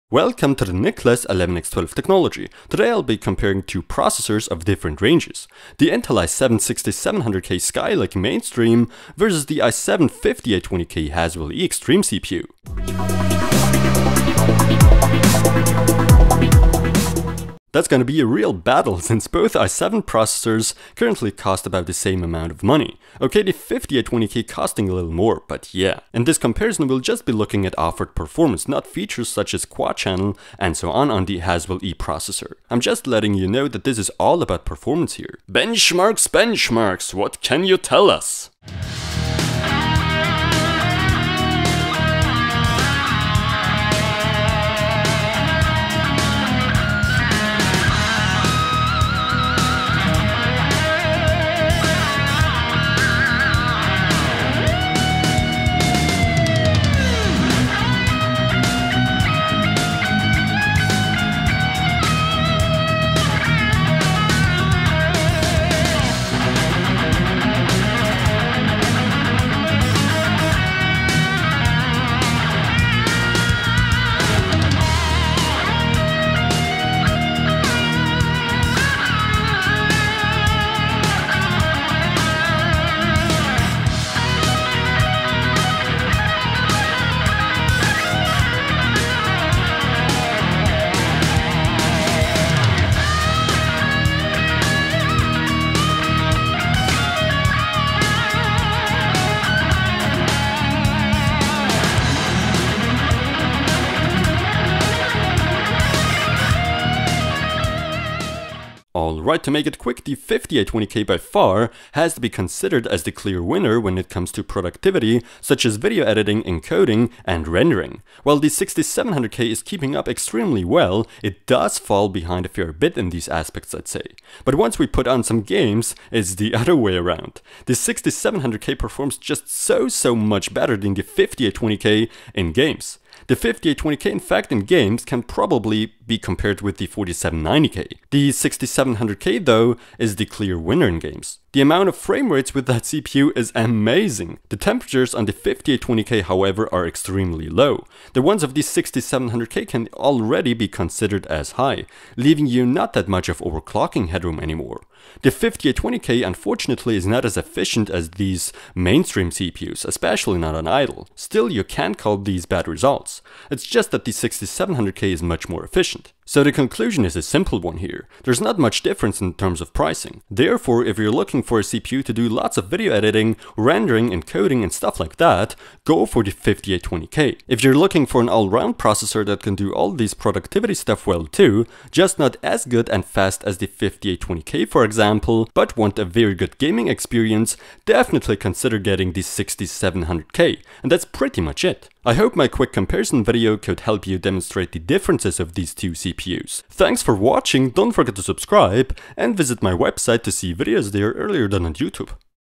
Intel i7-6700K vs i7-5820K
Nicolas11x12 comparing the Intel i7-6700K with the i7-5820K Processor.
How well do these CPUs perform?